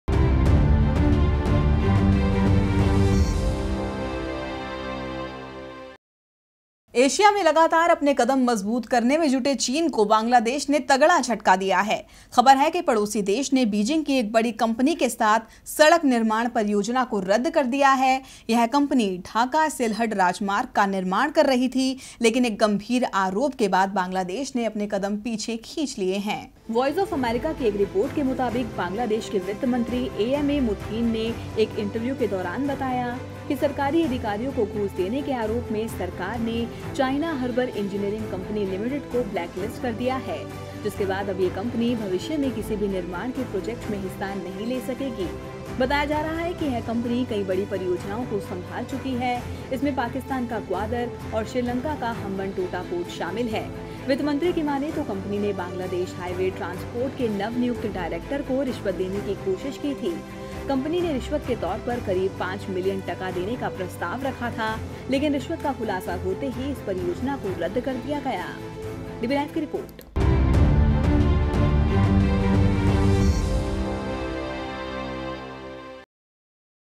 China को Bangladesh ने दिया झटका, China कंपनी को सड़क निर्माण करने से रोका
- एशिया में लगातार अपने कदम मजबूत करने में जुटे चीन को बांग्लादेश ने तगड़ा झटका दिया है। खबर है कि पड़ोसी देश ने बीजिंग की एक बड़ी कंपनी के साथ सड़क निर्माण परियोजना को रद्द कर दिया है। यह कंपनी ढाका सिलहट राजमार्ग का निर्माण कर रही थी, लेकिन एक गंभीर आरोप के बाद बांग्लदेश ने अपने कदम पीछे खींच लिए है